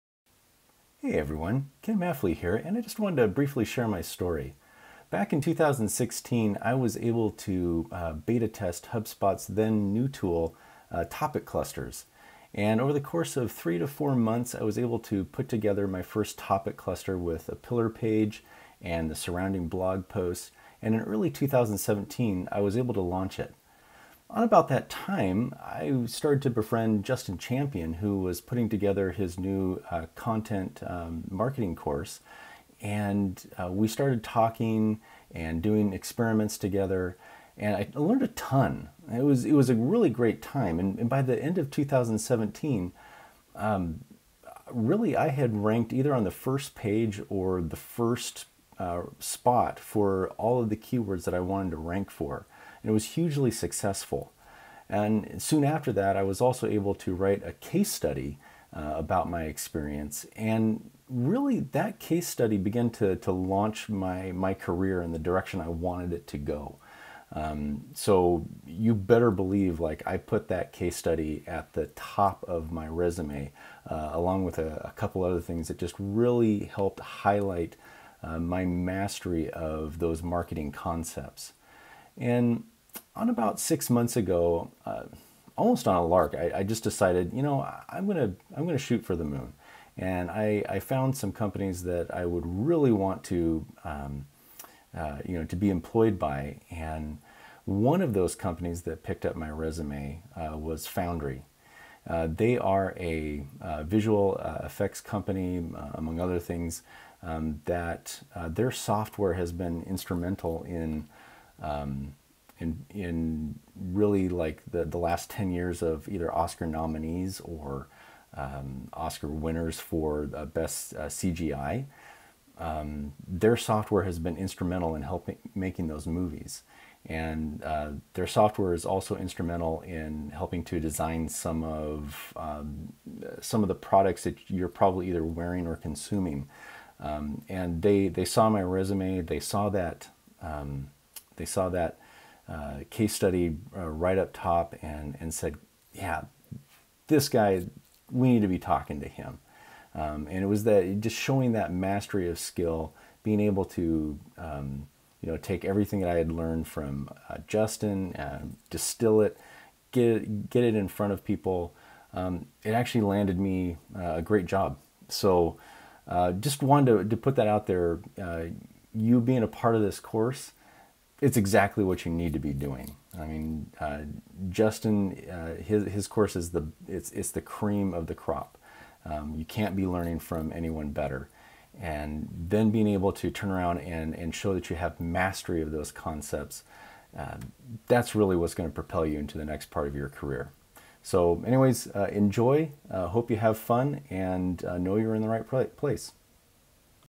Inbound Content Strategy Success Story - Ken Mafli
Hear a career success story directly from an inbound content strategist who has learned from and applied the techniques taught by HubSpot's principal inbound professor, Justin Champion.

Want to master your own inbound content strategy?